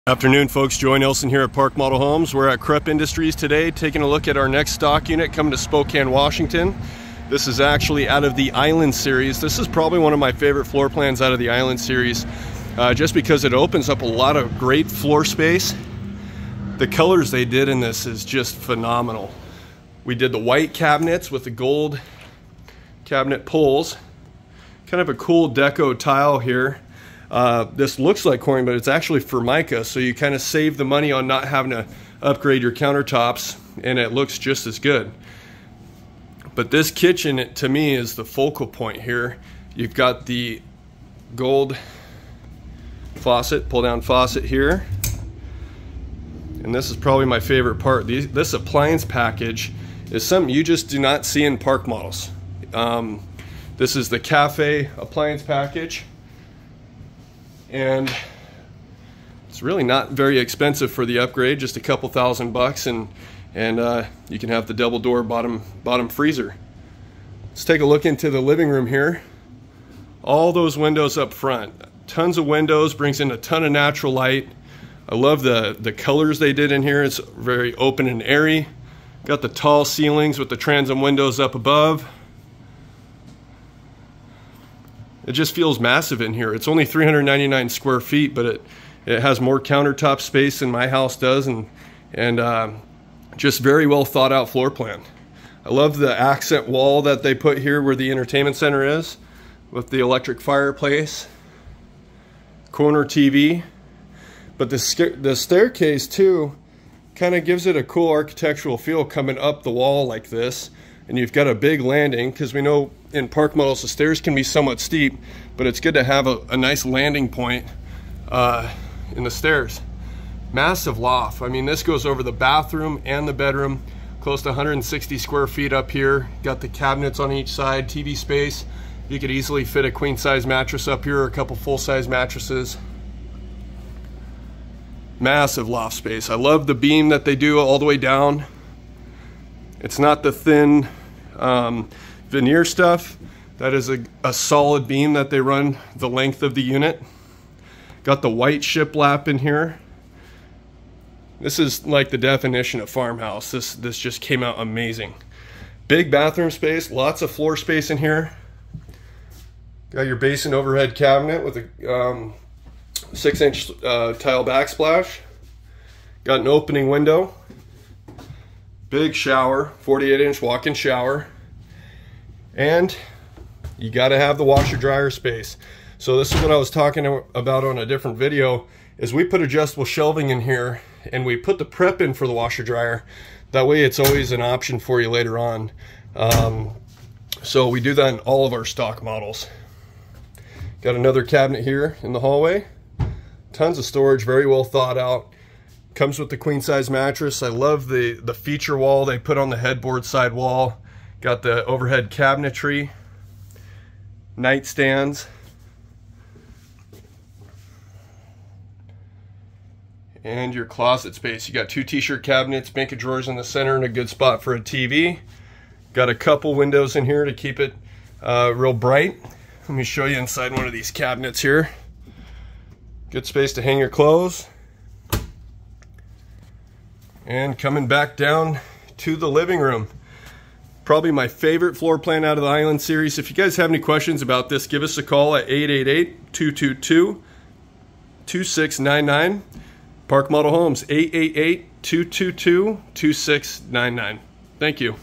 Unbelievable Tiny Home design that will leave you speechless!
Its a fantastic day here at Park Model Homes! We are featuring for the first time the new 2024 Kropf Cafe model! This home has so much character this one will leave you speechless. The outside exterior is loaded with a beautiful window package that will bring in a ton of natural. We love the outside shake accent you just don't see manufactures doing right now. Once you walk inside the first thing, you're going to notice is how beautiful the kitchen is with the white cabinets and gold hardware accents. This is the first time we have ever featured the cafe appliance package in any of our units and we knew it was going to be a showstopper! There have been countless hours in research and design with the Kropf team and they hit the nail on the head with this one!